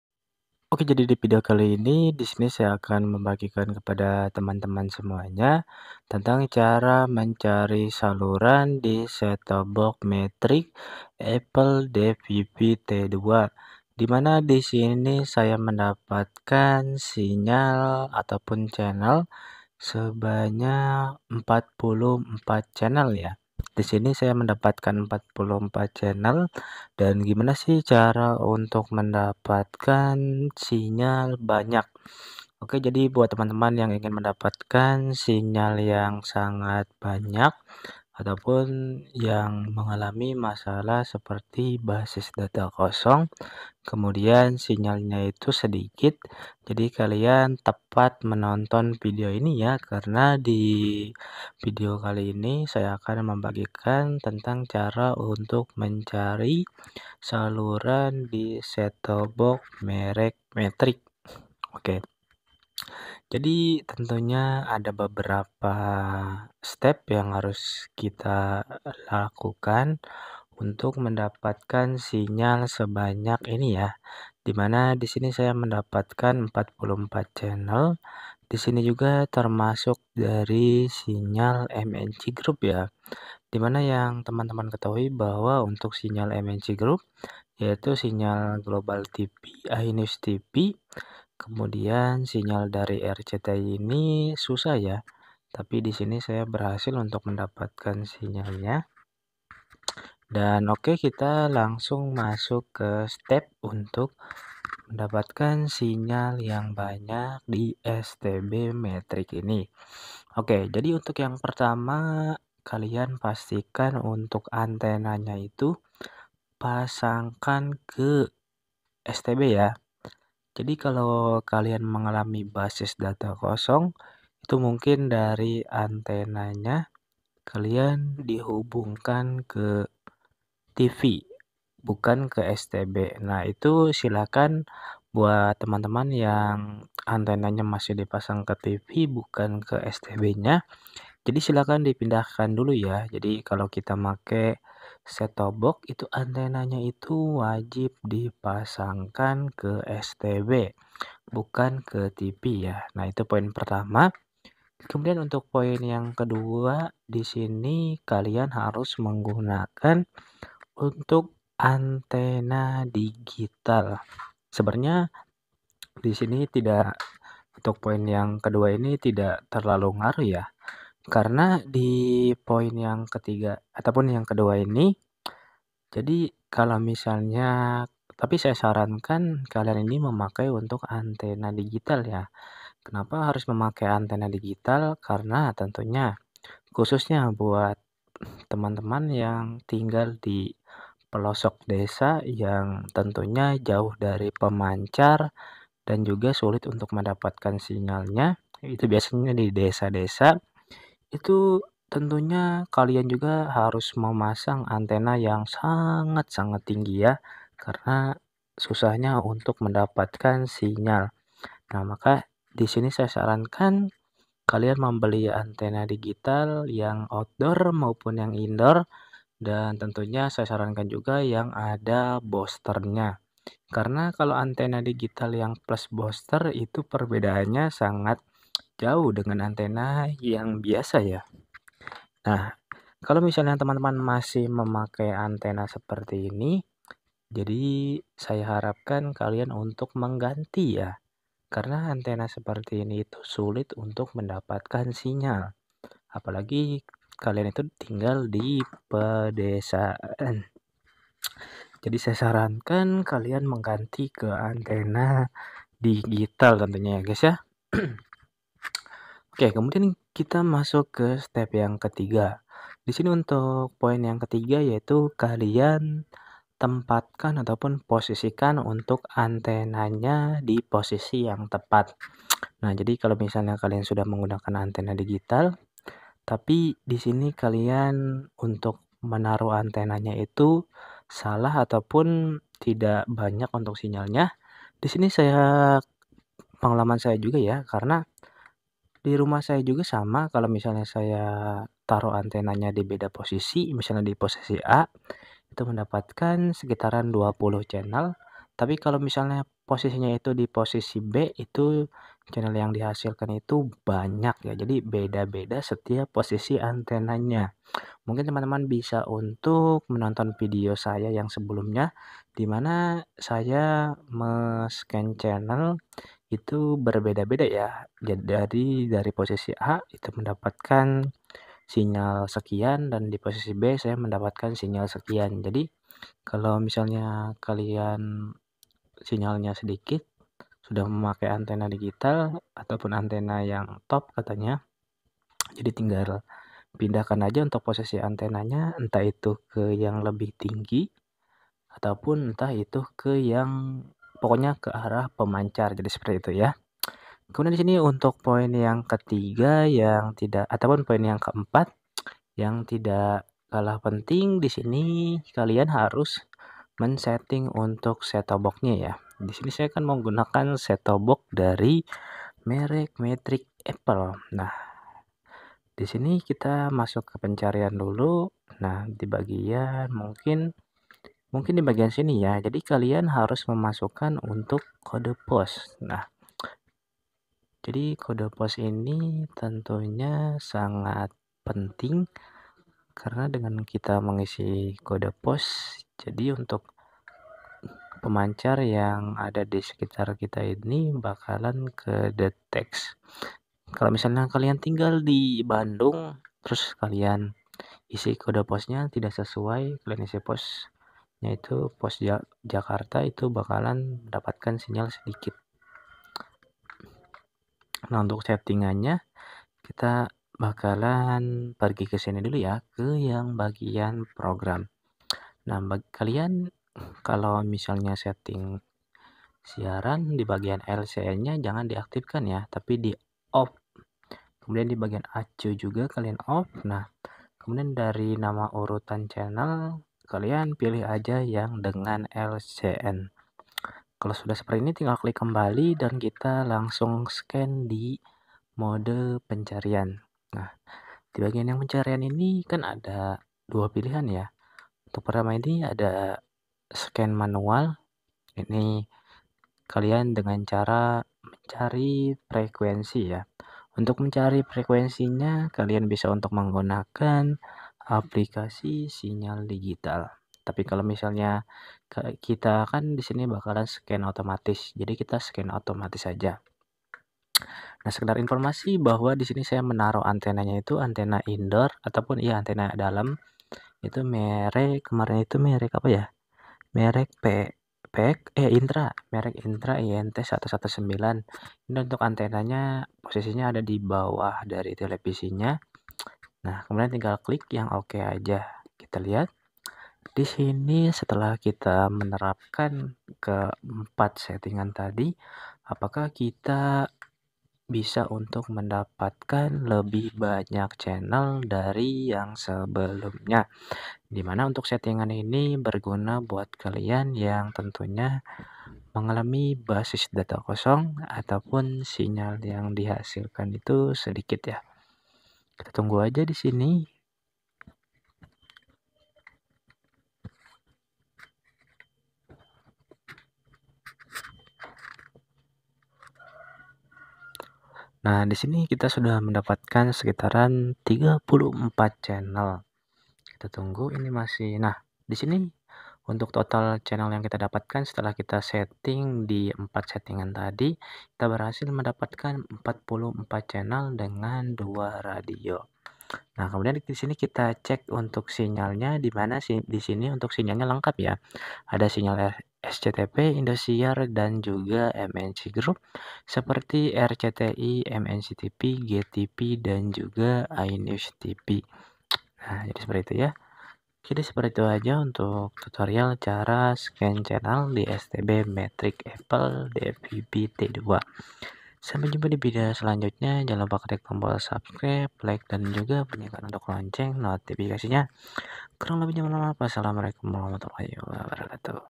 4 Tips Cara Mencari Saluran Tv Digital Di Set Top Box | Scan Channel Di Set Top Box
Buat kamu yang kesulitan untuk mendapatkan channel di set top box, silahkan di tonton untuk video ini karena aku di sini membagikan tentang cara mencari saluran tv digital di set top box, jadi buat kalian yang kemarin tidak mendapatkan sinyal atau bahkan basic data kosong ,dan juga sinyal yang di hasilkan sedikit, silahkan di simak karena saya membagikan 4 tips cara untuk mendapatkan saluran di set top box.